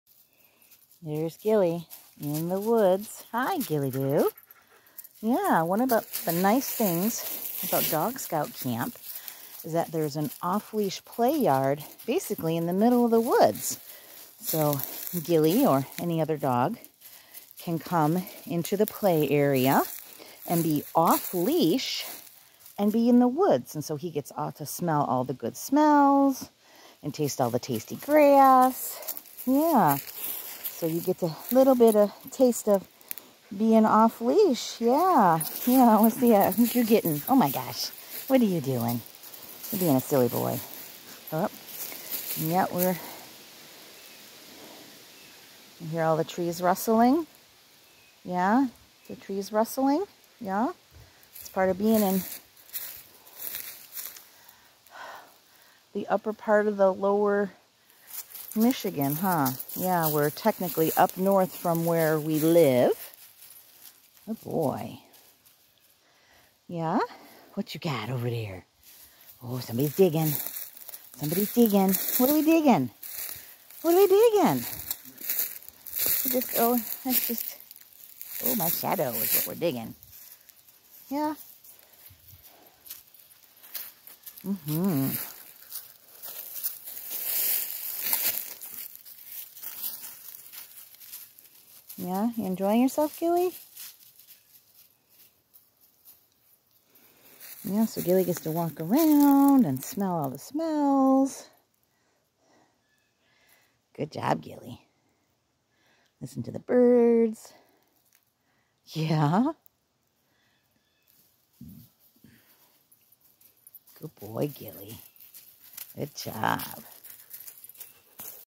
Husky Off Leash in the Woods (in a secure area)
Ghillie Dhu explores the off leash yard at Dog Scout Camp.